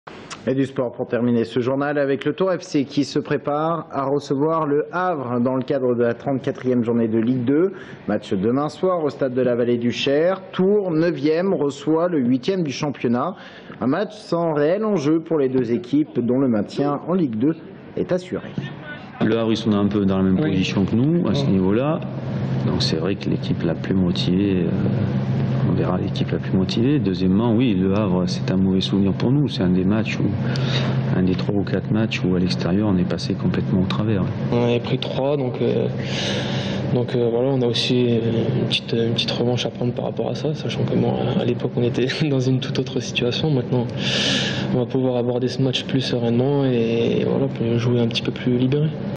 Foot / Ligue 2: Tours - Le Havre (l'avant-match)
Sites webs/blogs, copiez cette vidéo avec le bouton "Intégrer" !
 Les footballeurs du Tours FC se préparent à affronter Le Havre dans le cadre de la 34ème journée de Ligue 2.